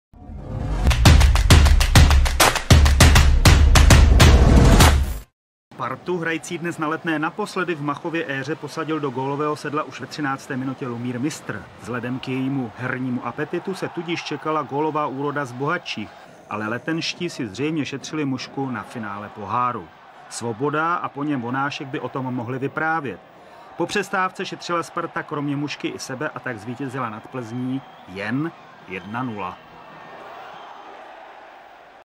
18/5/1996 Sparta Praha - Viktoria Plzeň 1-0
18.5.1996
Gambrinus liga 1995/96 - 30. kolo
Sparta Praha - Viktoria Plzeň 1-0
Branka: 13. Lumír Mistr
Diváci: 4228

Sparta: Petr Kouba - Michal Horňák, Jiří Novotný, Tomáš Řepka (42. Miroslav Rada) - Roman Vonášek, Lumír Mistr, Václav Budka (54. Antonín Plachý), Zdeněk Svoboda, Martin Frýdek, Pavel Nedvěd - Vratislav Lokvenc (46. Josef Obajdin). Trenér: Vlastimil Petržela

Plzeň: Michal Čaloun - Petr Vlček, Miloš Slabý, Libor Čihák, Zdeněk Bečka - Michal Drahorád, Stanislav Purkart, Jiří Skála, Martin Kulhánek (85. Vladimír Myslík) - Patrik Ježek, Václav Ušák (82. Tomáš Heřman). Trenér: Jaroslav Hřebík